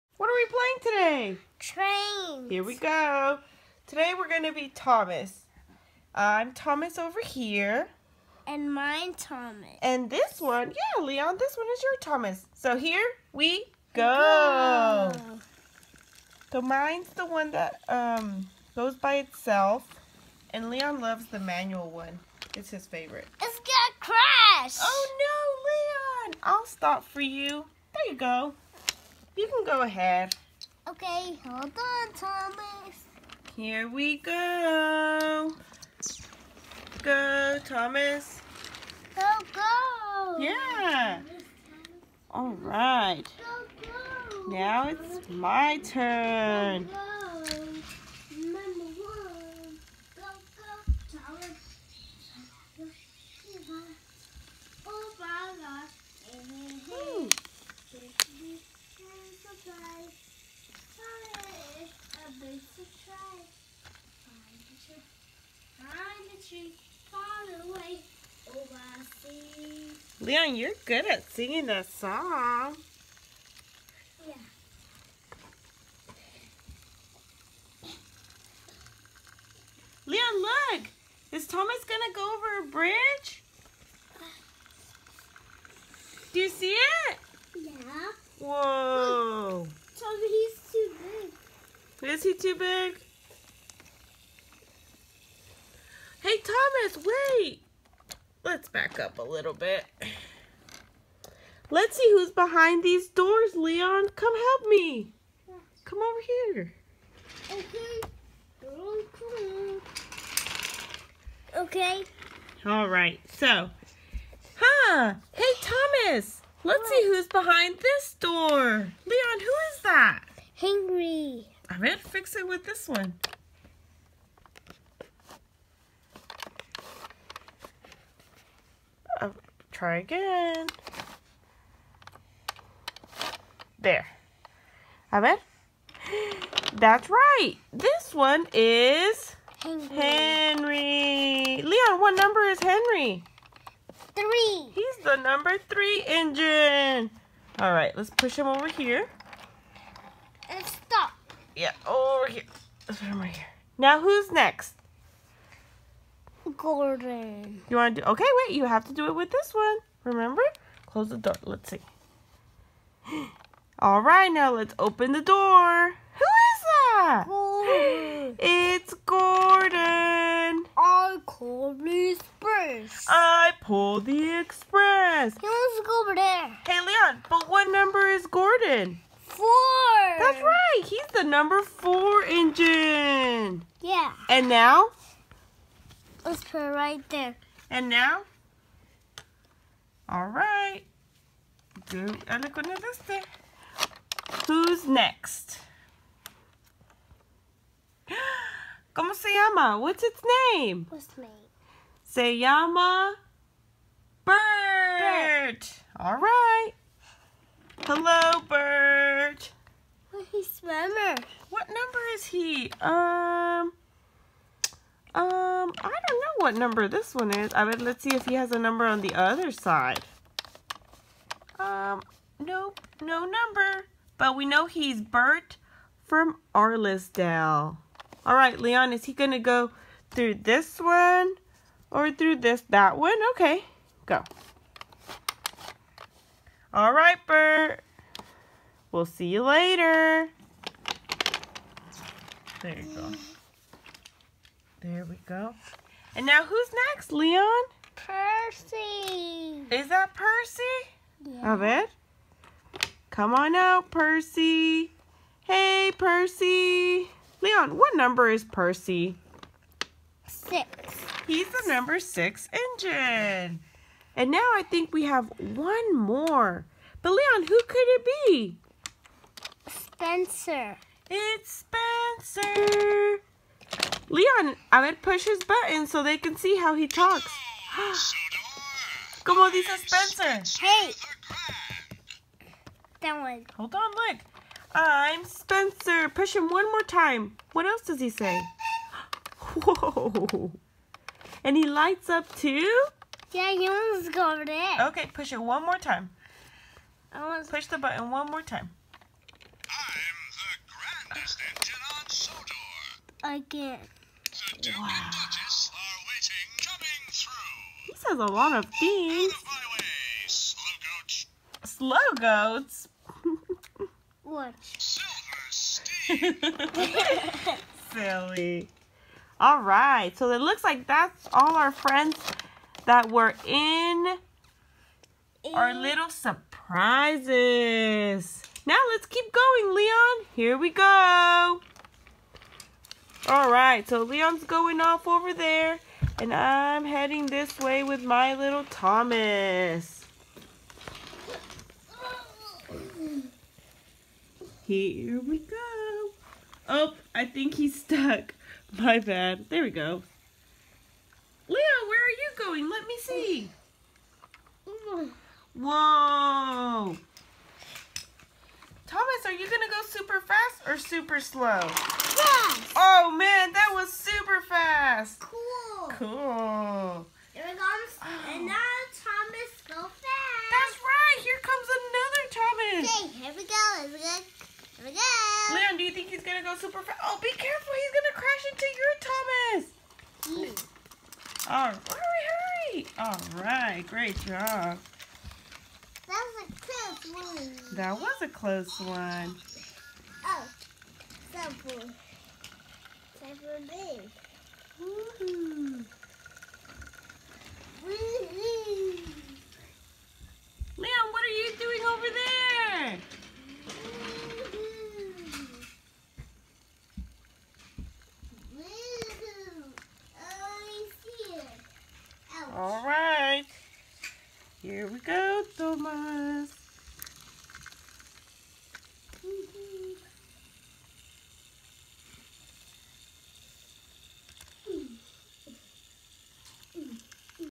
Thomas and Friends adventure with LEON! Wooden Railway Tidmouth Shed
Leon LOVES his Tidmouth Shed from Fisher-Price!